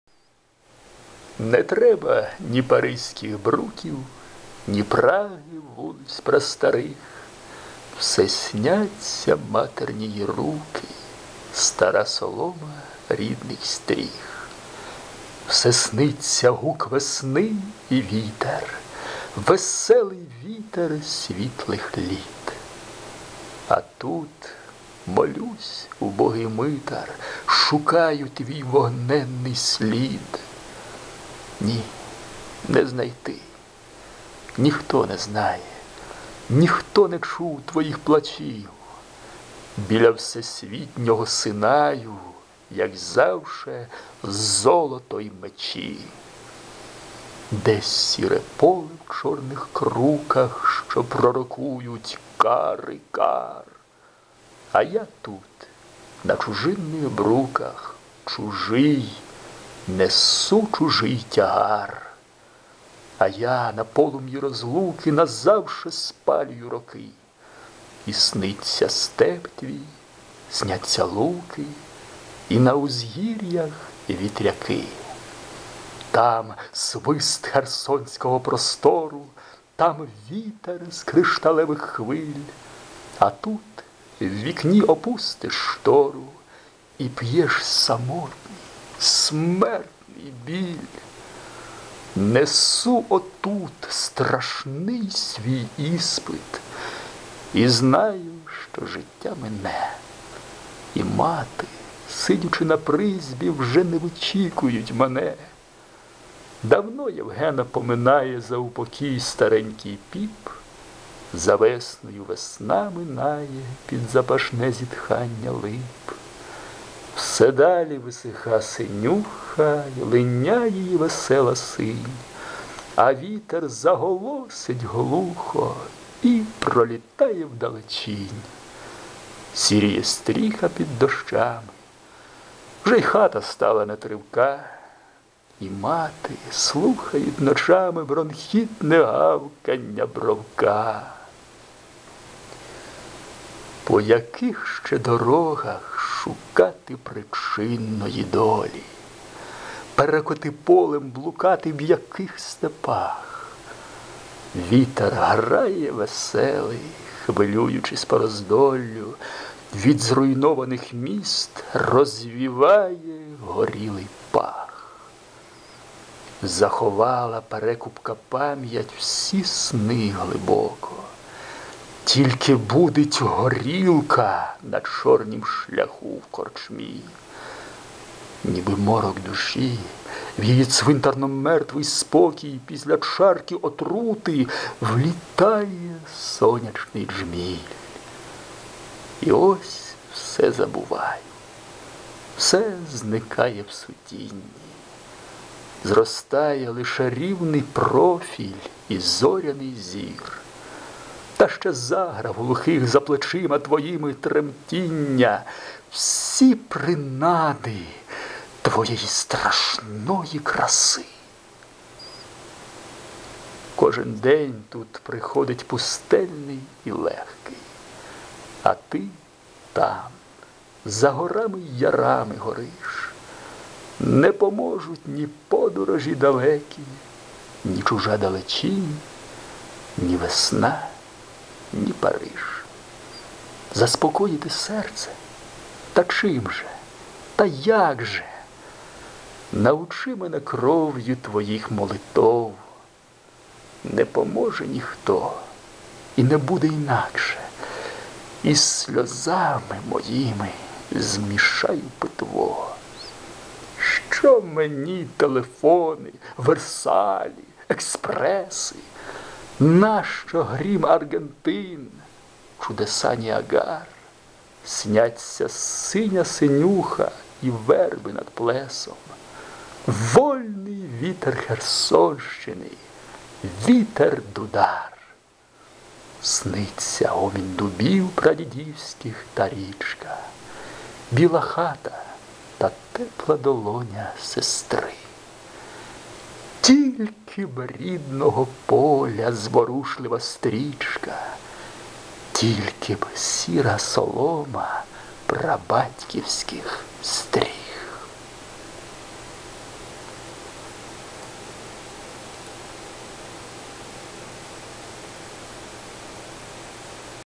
Євген Маланюк - ПІД ЧУЖИМ НЕБОМ
Читає Василь Білоцерківський.